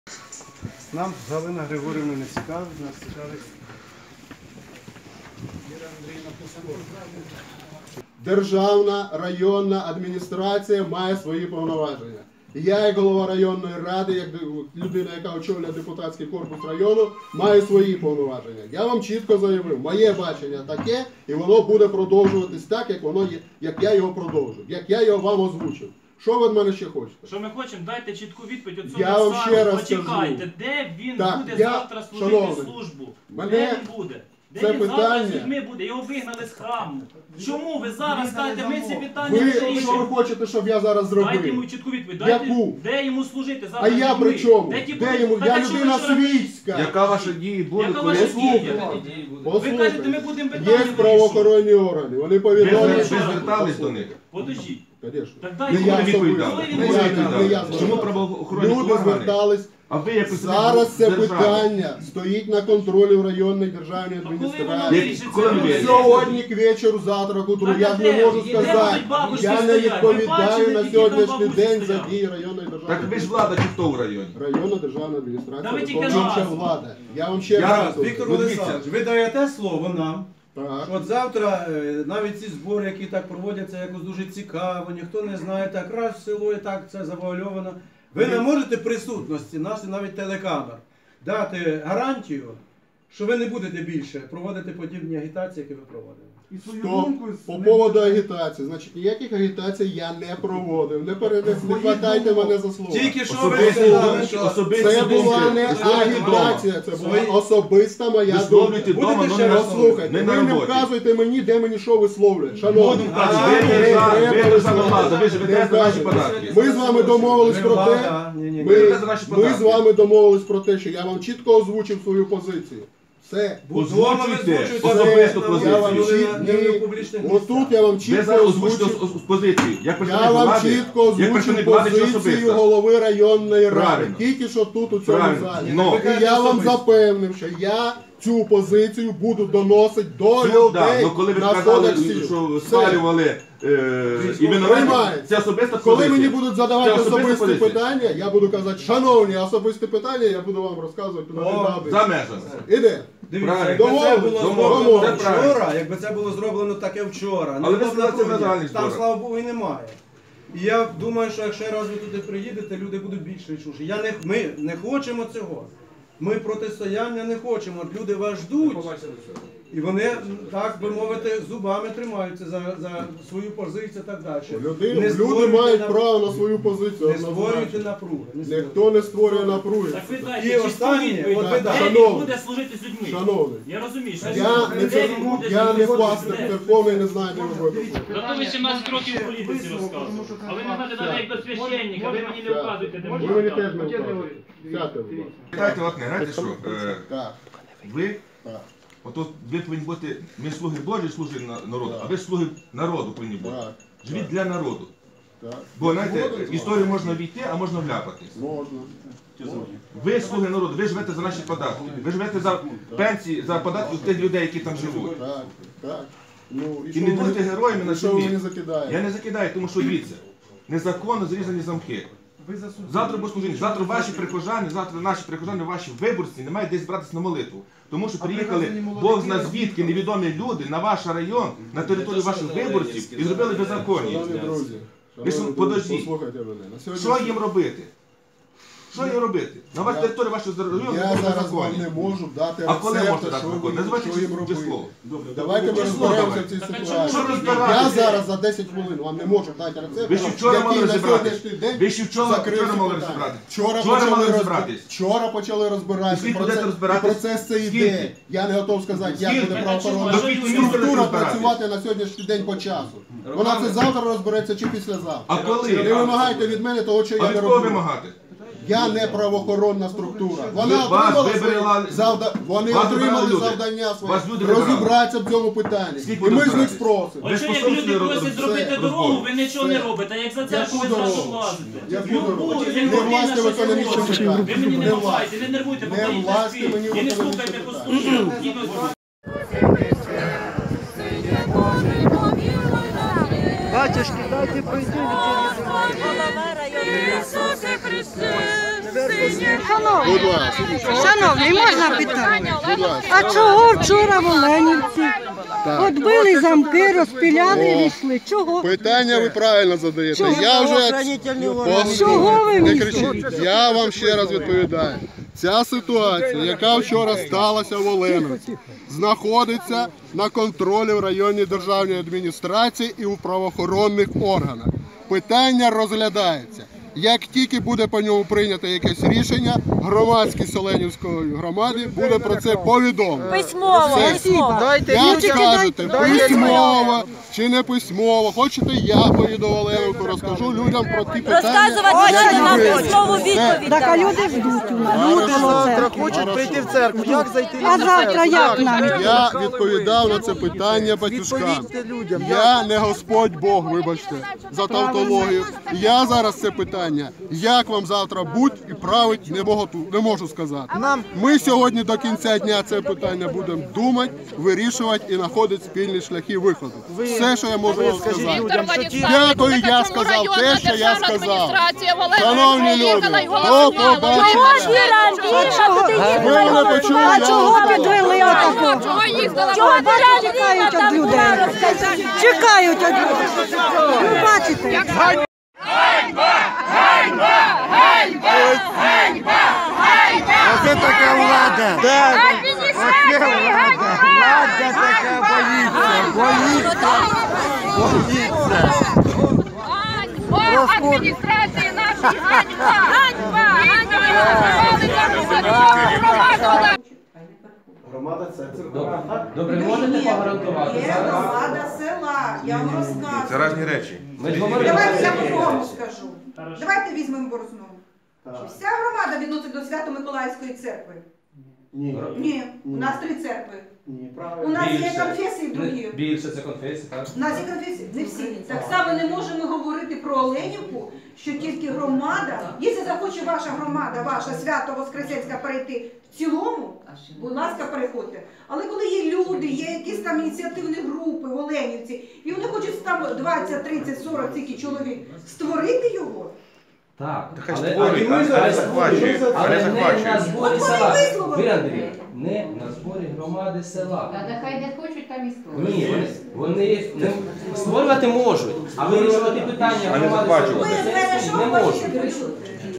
Громадськість Чернігівщини вимагає від влади припинити агітацію чиновників за перехід в "ПЦУ"
17 січня 2019 р. громадськість та духовенство  м. Борзна та Борзнянського району вимагала від Борзнянської РДА та районної ради народних депутатів припинити агітацію чиновників за перехід в «ПЦУ».